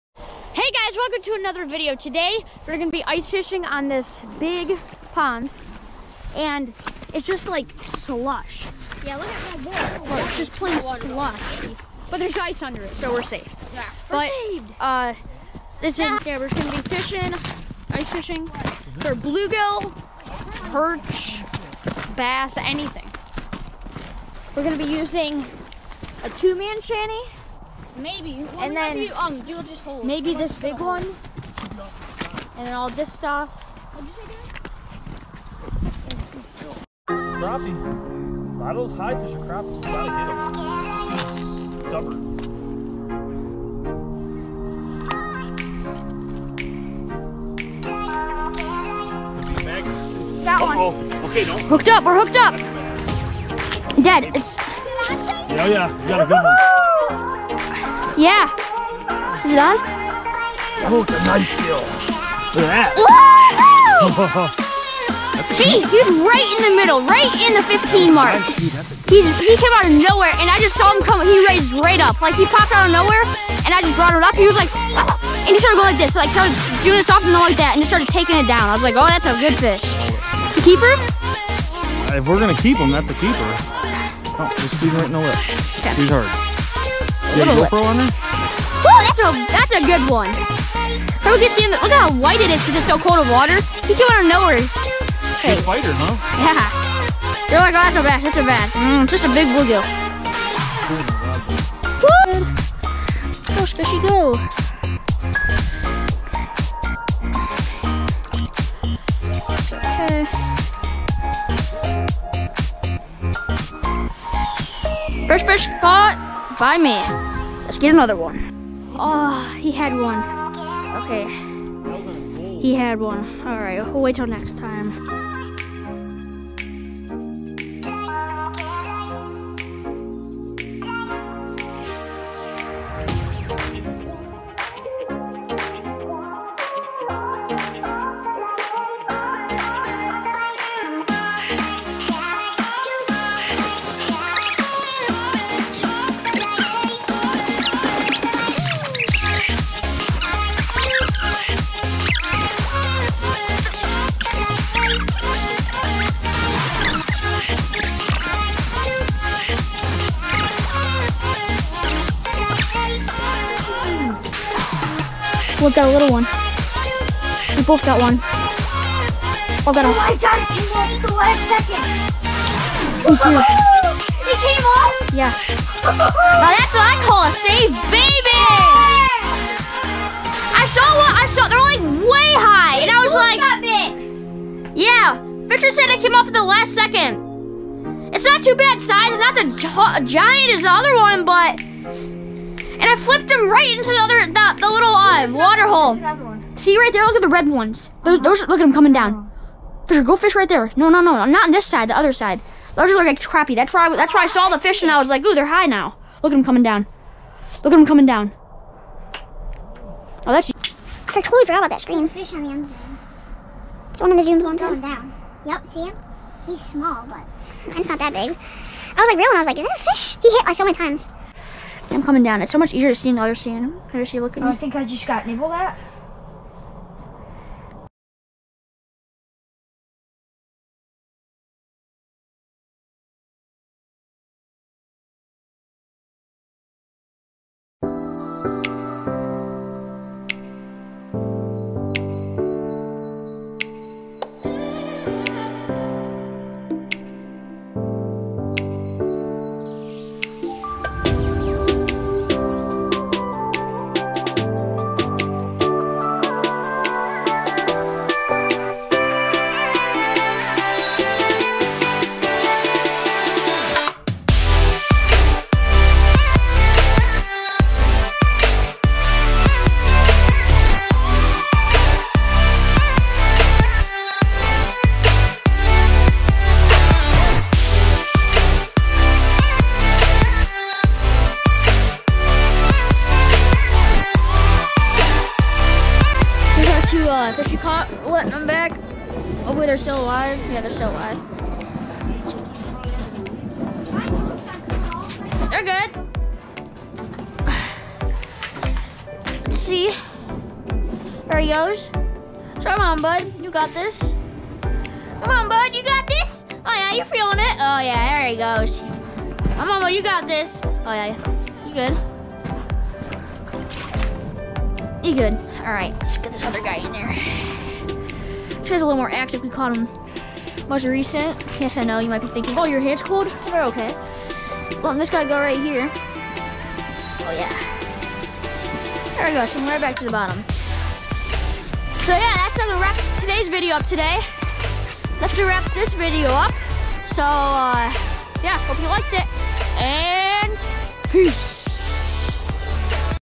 Ice Fishing Some Big Bluegill
We will be back and we will try new spots.  There are giant catfish in the pond.  Never caught one but there are some there.  There are giant bass there my personal best there is 7 pounds.  There are way bigger ones in there, my dad had a 3 pounder on and it was 5 feet from shore and then a monster came and ate it.  He had it on for about 5 minutes and then it snapped.  Swallowed whole that 3 pound bass was never seen again.   THE END.🙂